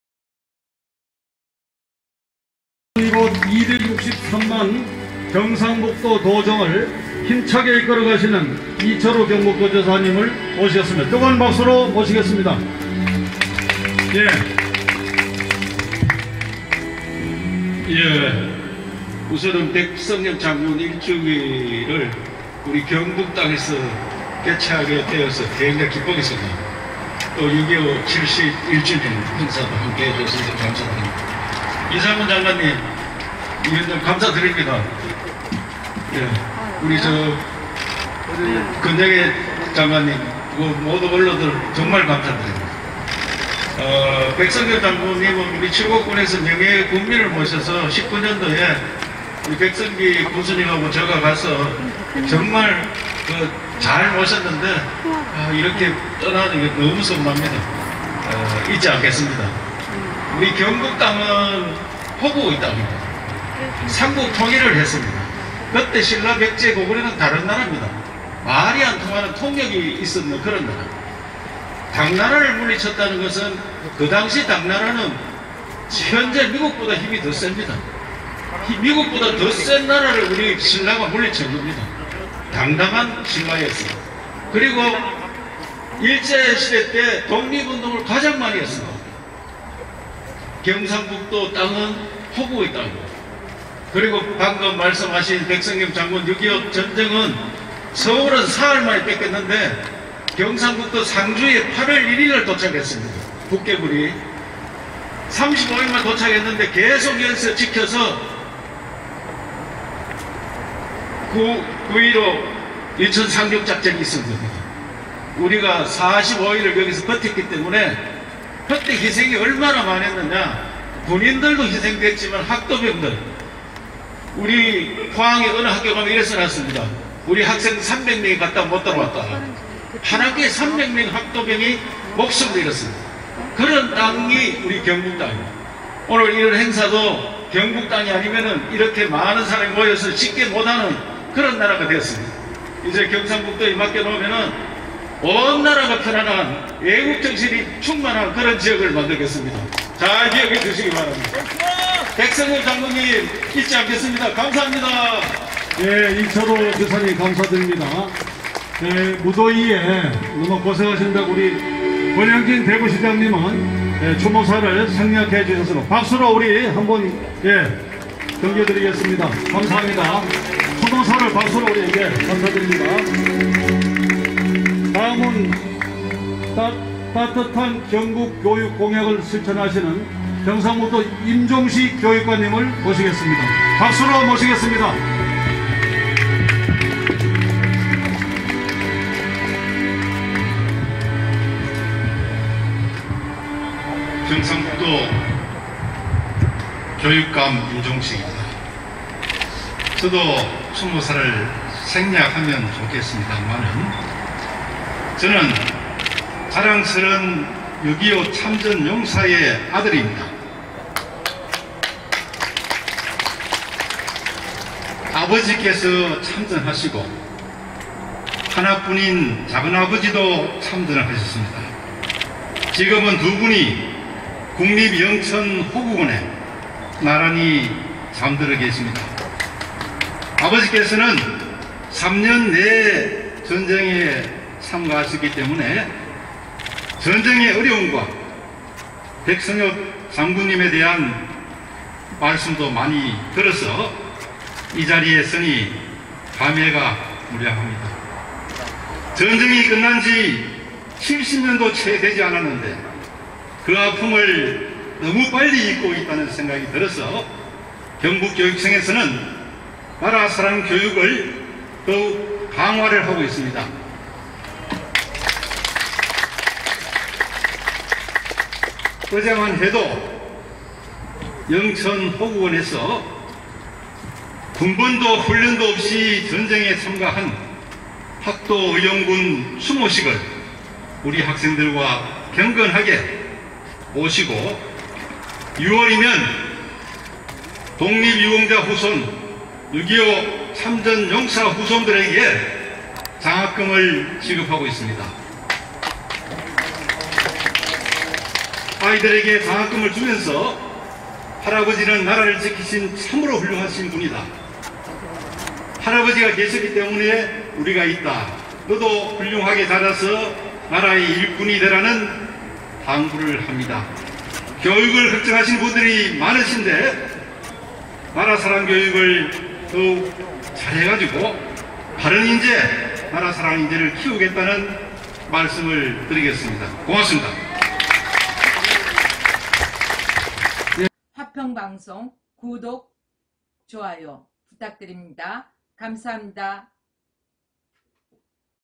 [추모사] 이철우 도지사 (경상북도) 임종식 교육감 (경상북도) / 6.25전쟁 상기 71주년 행사 백선엽 장군 서거 1주기 추모행사 - 화평방송
등록번호 인천,  아01406